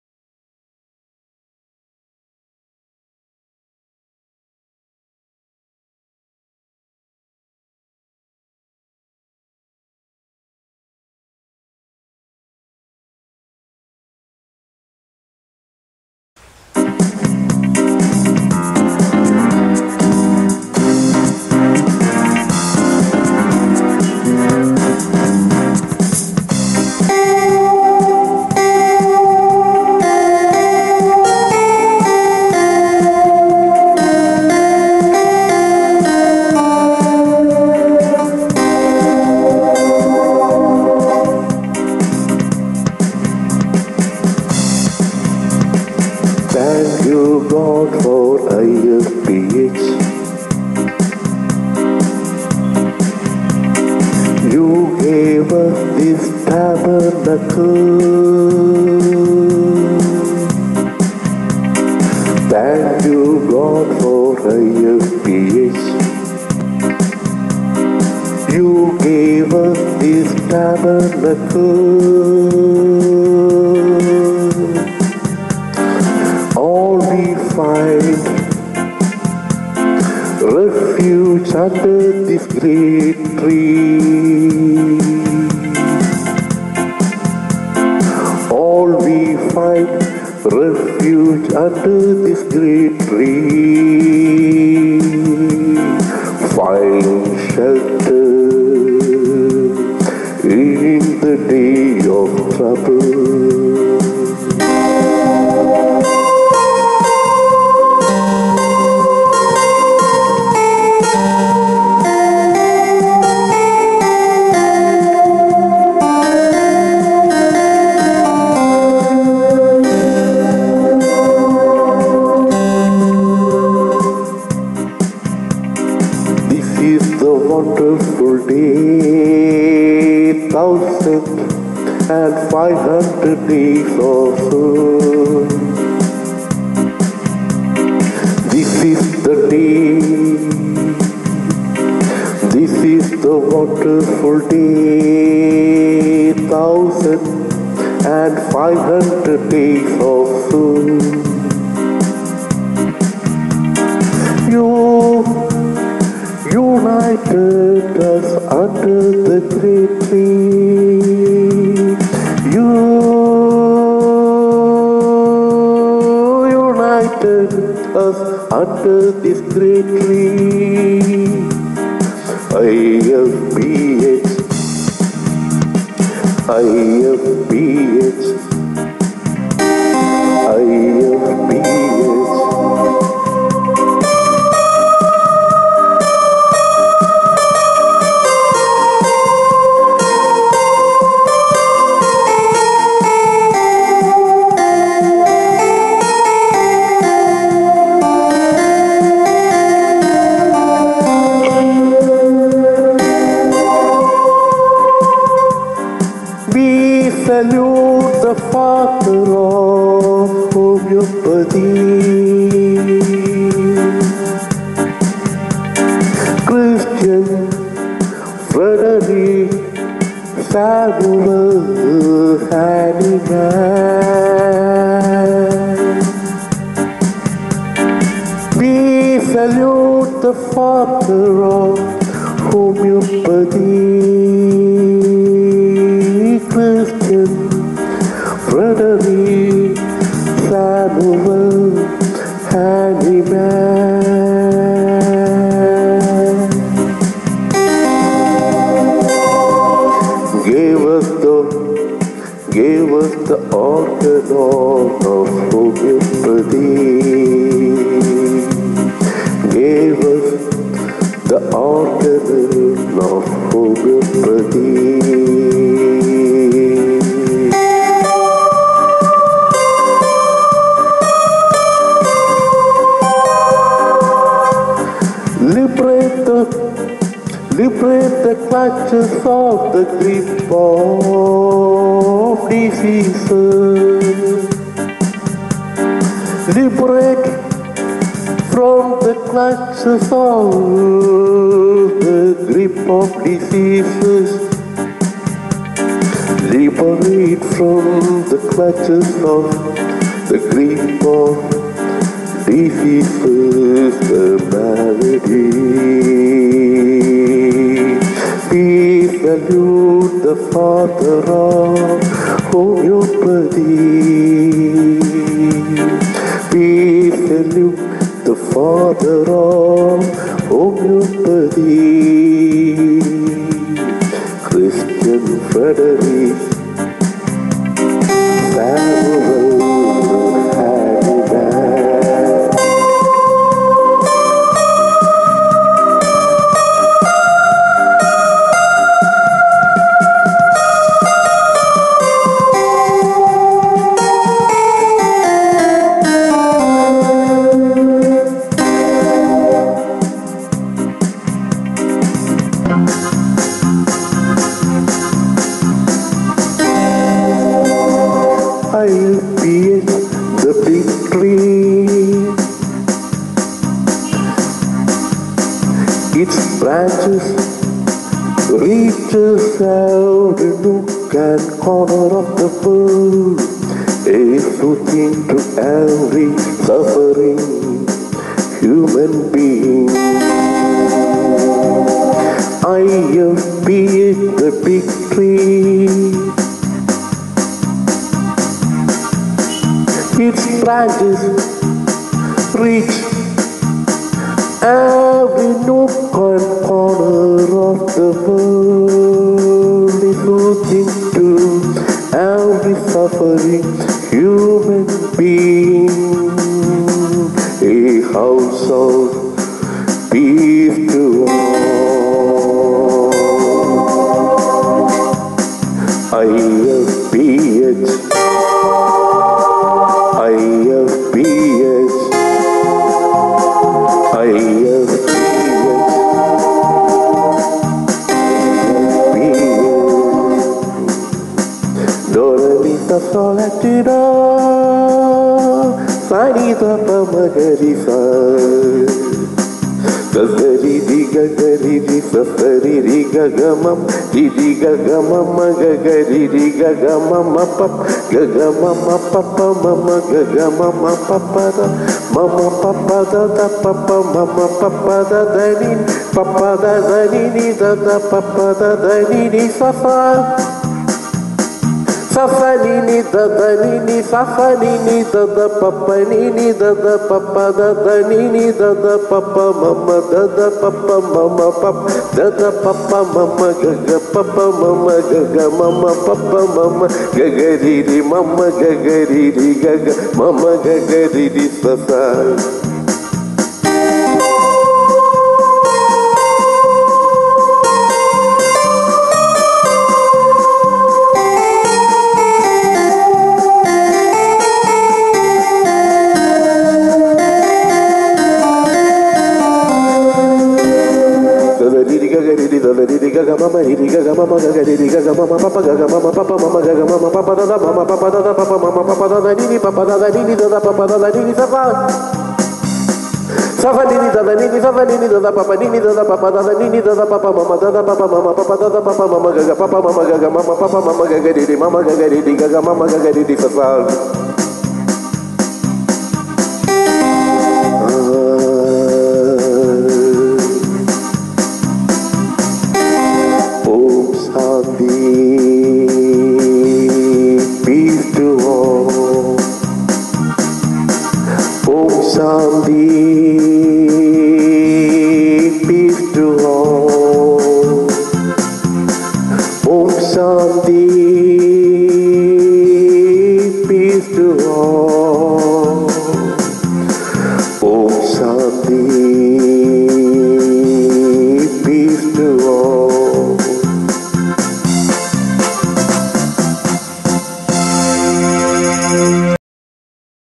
INTERNATIONAL FORUM FOR PROMOTING HOMOEOPATHY [IFPH] -1500 DAYS COMPLETED - WORLD RECORD SONG
ഹോമിയോപ്പതി വൈദ്യ ശാസ്തത്തിന്റെ ലോകം മൊത്തമുള്ള വളർച്ചയ്ക്കായി ദയവായി, സബ്സ്ക്രൈബ് ചെയ്യുക, ഷെയർ ചെയ്യുക

1500th DAY CELEBRATIONS OF IFPH WEBINAR -DAY 5  English] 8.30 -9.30 pm